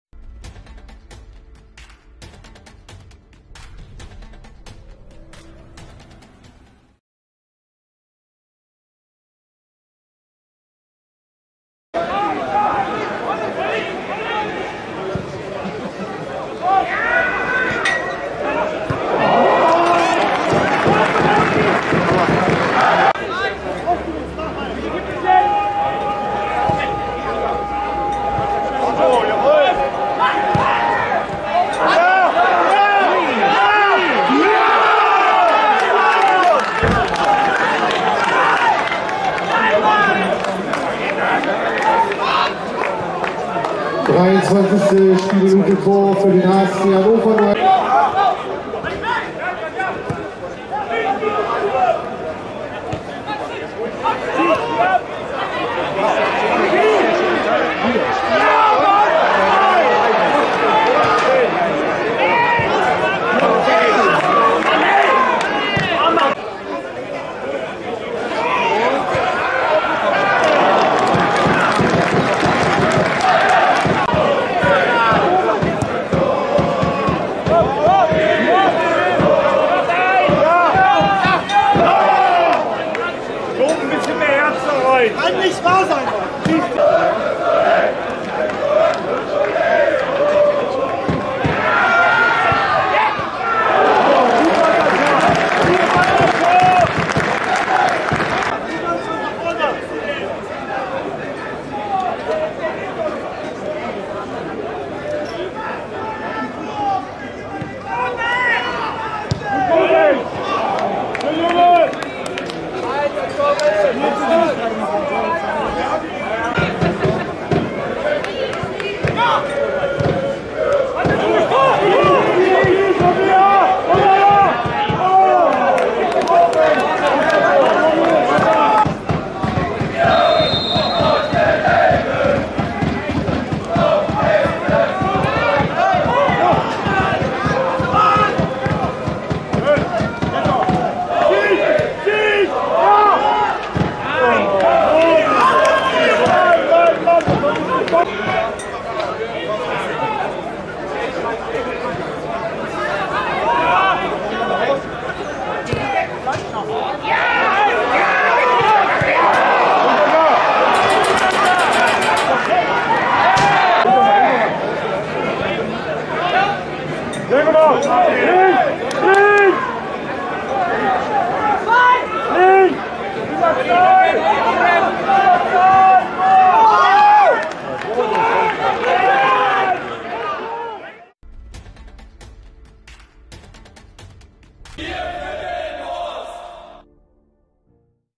SV Atlas - HSC Hannover (0:1)
Heimniederlage für unseren SV Atlas gegen den HSC Hannover.